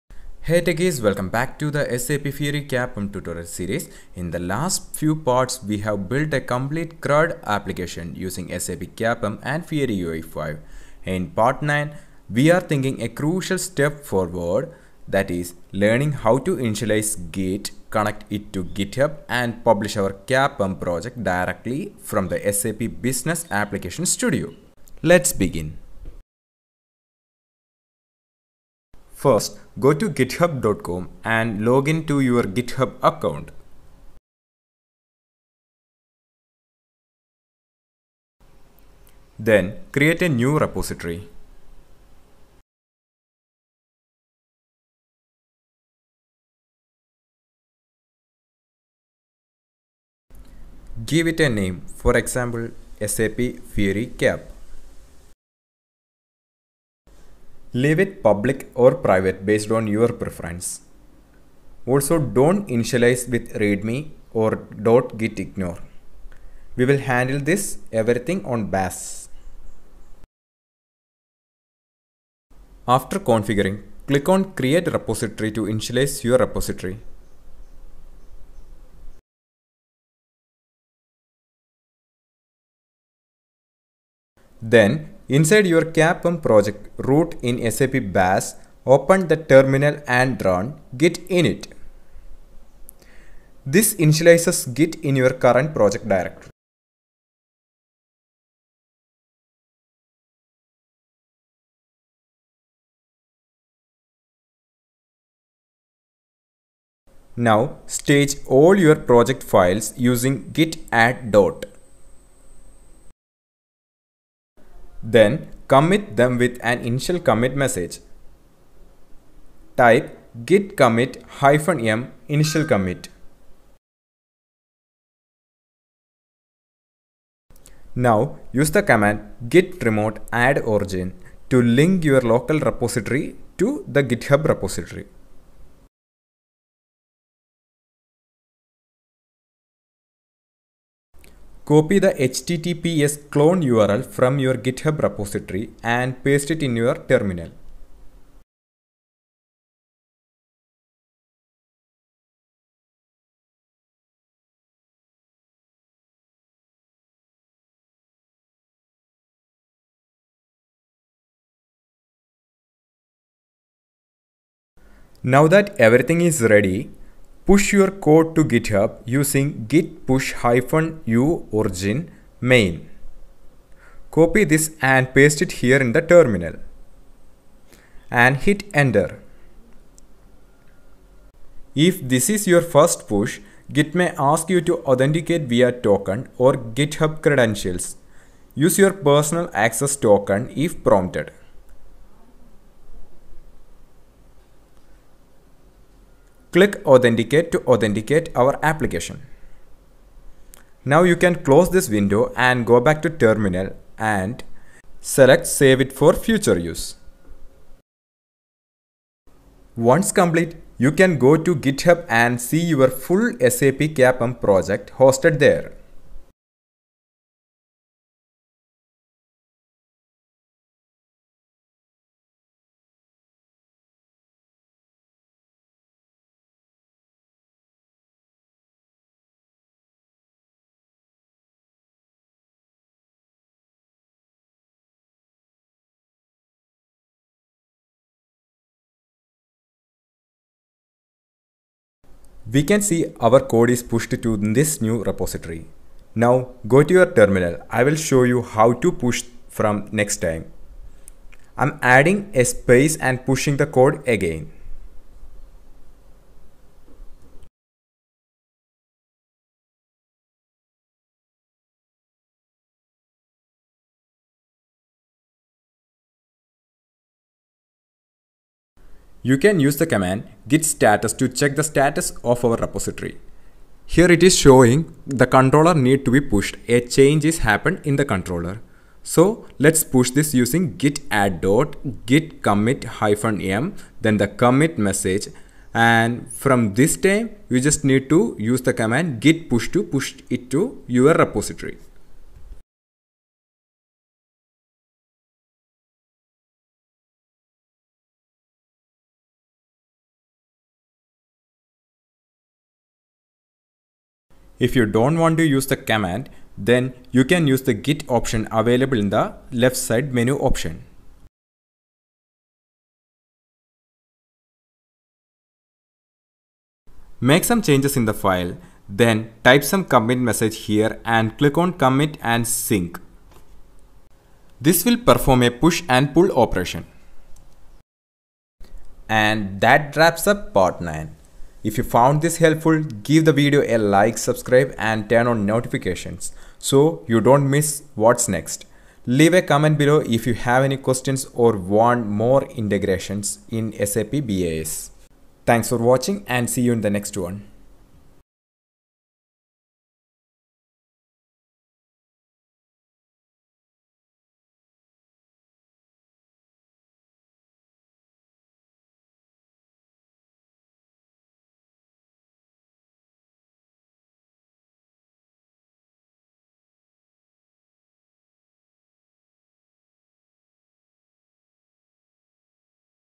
SAP Fiori CAPM Tutorial Part 9: Initialize Git and Push Project to GitHub from SAP BAS
In Part 9 of our SAP CAPM & Fiori UI5 tutorial series, we focus on setting up version control for your project by initializing Git, connecting to GitHub, and pushing your CAPM project from SAP Business Application Studio (BAS).

Version control is essential for team collaboration, backup, and deployment workflows. In this video, you'll learn how to easily publish your project and keep your development organized using Git.

🛠️ What You'll Learn:
📌 How to initialize a Git repository in SAP BAS
📌 Stage and commit files with Git
📌 Connect to a GitHub remote repository
📌 Push your CAPM project to GitHub
📌 Best practices for version control in SAP development

🧰 Commands Used:
git init
git add .
git commit -m "initial commit"
git push -u origin main

📋 Before Watching This Video:
📌 Ensure you’ve completed the previous parts where we built a fully functional SAP CAPM app with:
📌 CRUD operations via OData V4
📌 A Fiori UI5 frontend integrated into the CAPM project
SQLite database connectivity for persistence